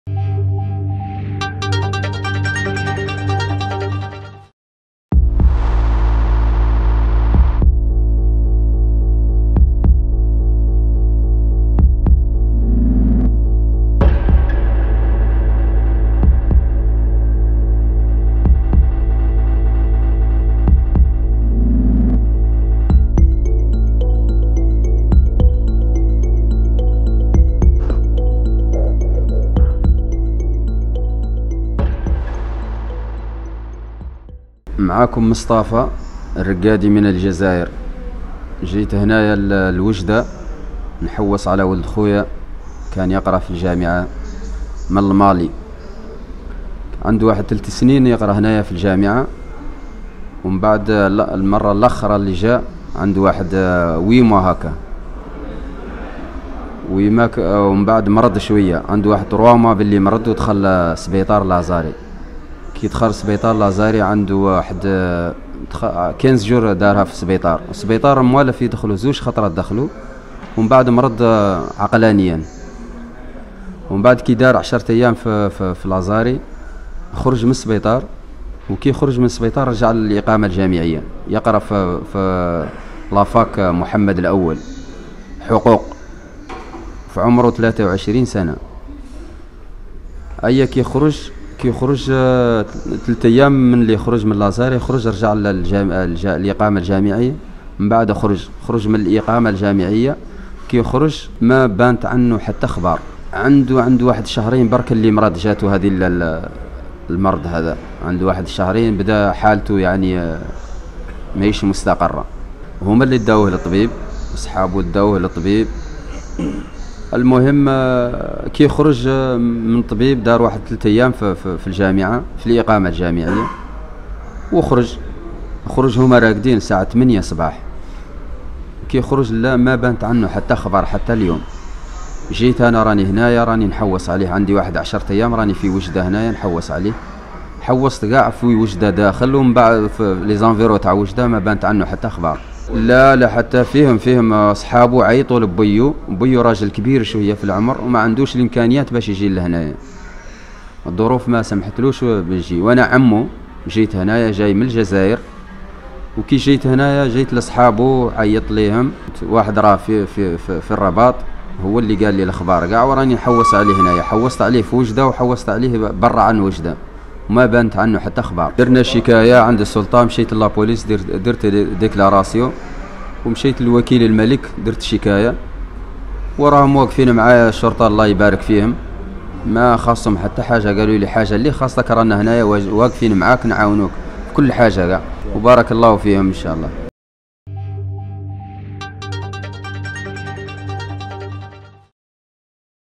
اختفاء طالب مالي بوجدة بطريقة غامضة..تفاصيل مثيرة وغريبة..شكون شافو؟
رصدت شوف تيفي تفاصيل مثيرة حول اختفاء طالب مالي في ظروف غامضة...للمساعدة:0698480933‎